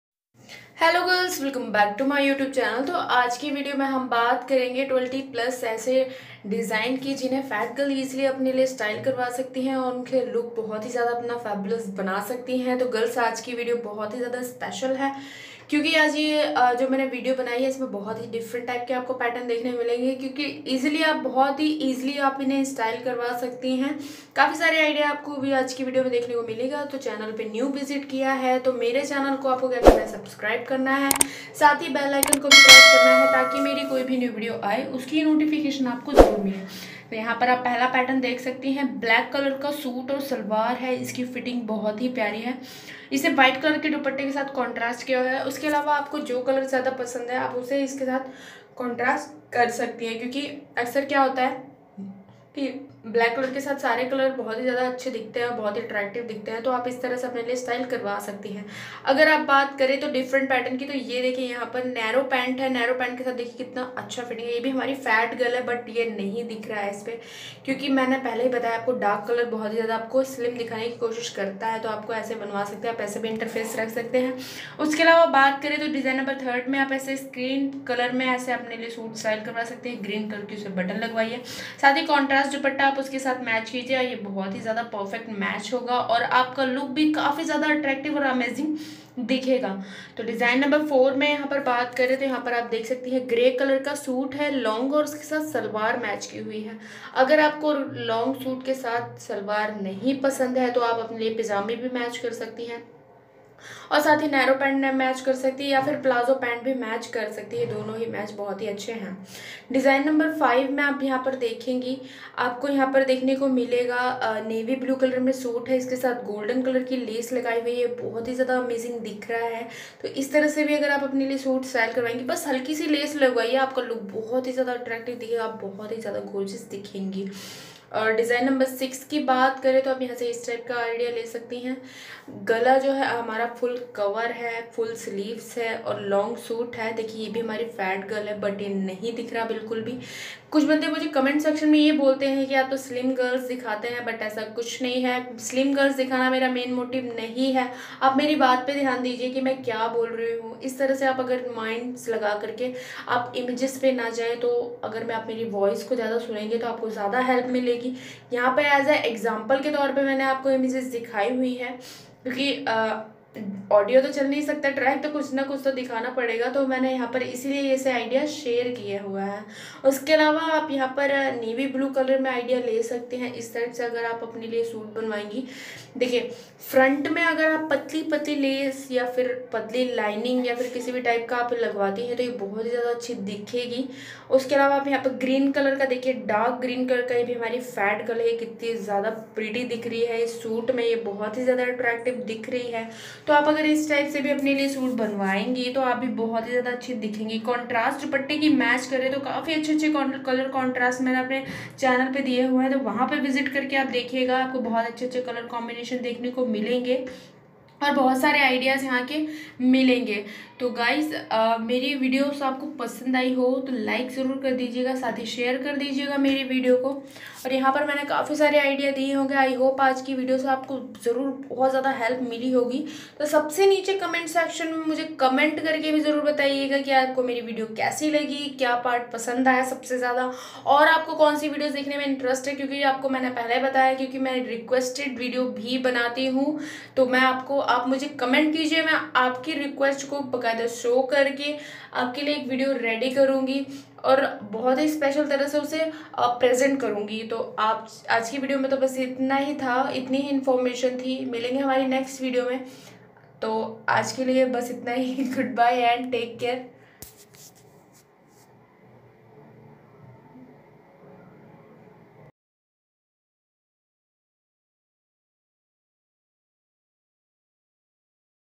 मोटी लड़कियां style करें अपने punjabi suit | grooming girl
Hello guys welcome back to my YouTube channel aaj ki is video me aap log dikhoge ki Maine yeha per bhaut sarevidea share kiye hai jin se apko bahut sare idea milenge sath hi apko new type ke color combination bhi dekhne ko milenge